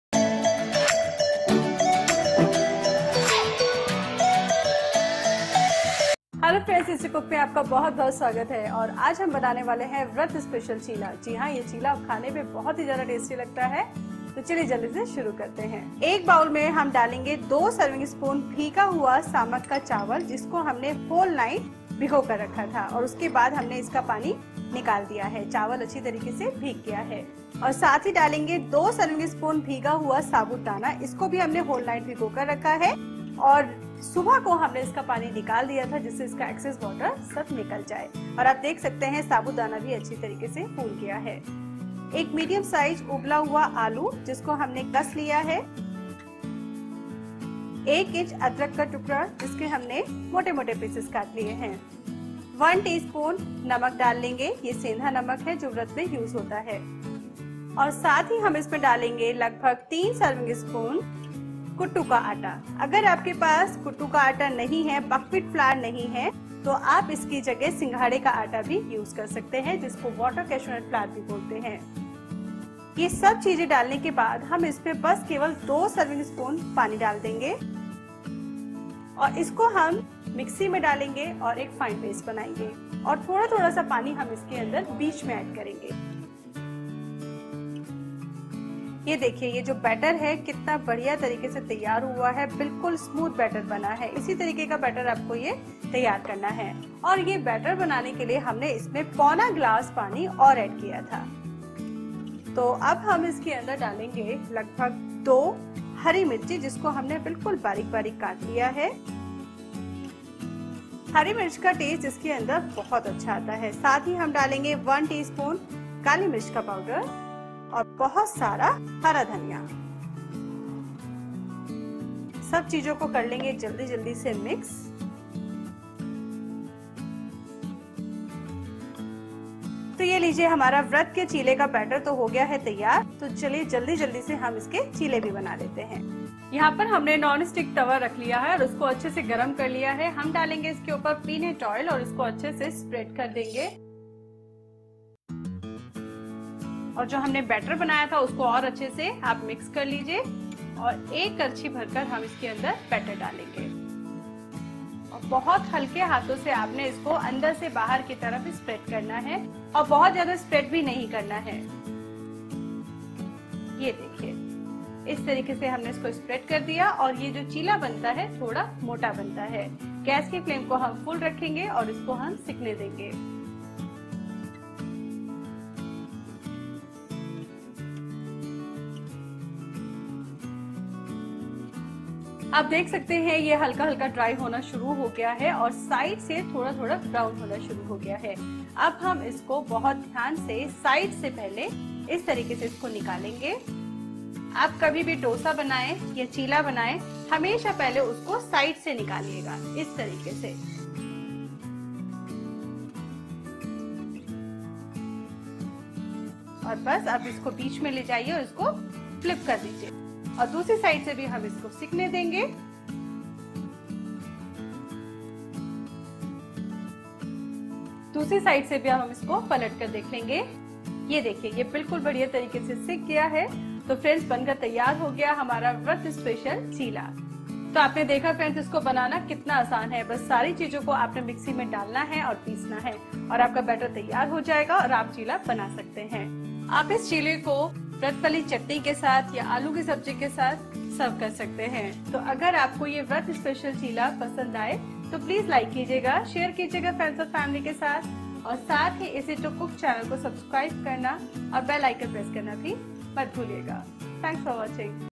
व्रत के खाने के लिए आसानी से बनने वाला व्रत स्पेशल चीला Vrat Cheela | Vrat Ka Khana
A special an easy recipe for Vrat. You can prepare it within 10 minutes.  This Chila is made with Potato, Sabudana (साबूदाना ) and Buckwheat flour (कुटु का आटा ). You can also use Water Cashew Nut flour (सिंघाड़े का आटा ) instead of Buckwheat flour.  You can prepare this recipe for Navaratri vrat also.

Eazy2Cook - Make Yourself a Perfect Cook
Some Special Deals on Kitchen Products.

Playlist of Amazing Recipes from Eazy2Cook